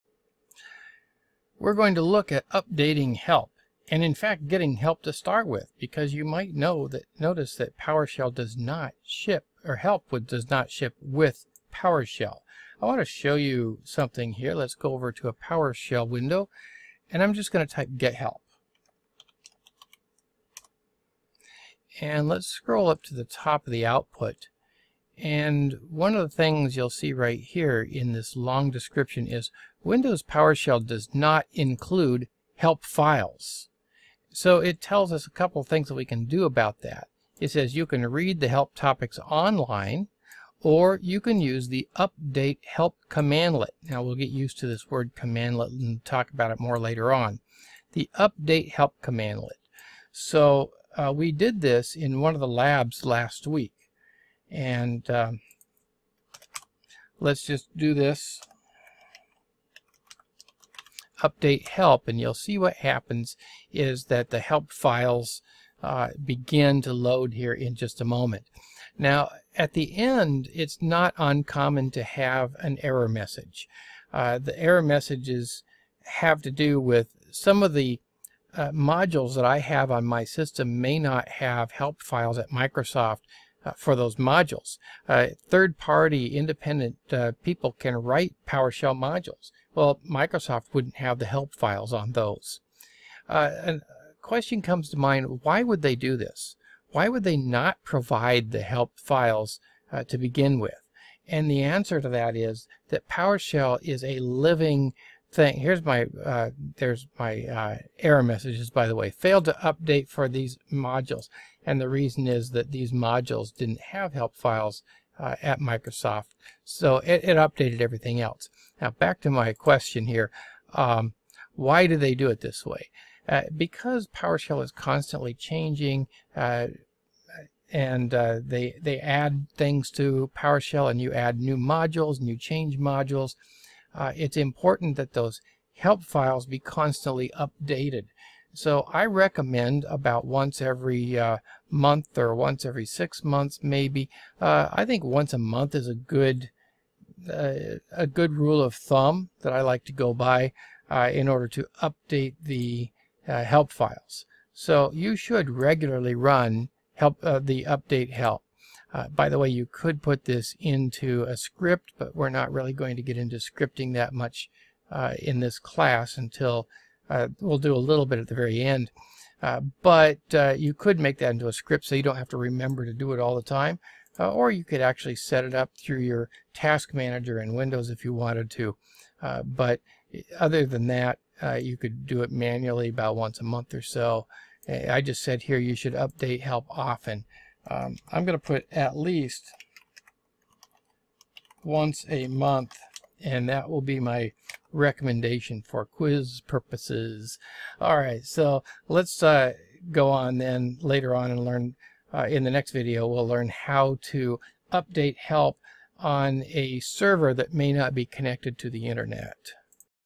Powershell: Getting and updating help files (Intro to Powershell series video 3-3)
This video is part of a classroom series on PowerShell. It is meant to be viewed as part of the class notes, but is publicly available.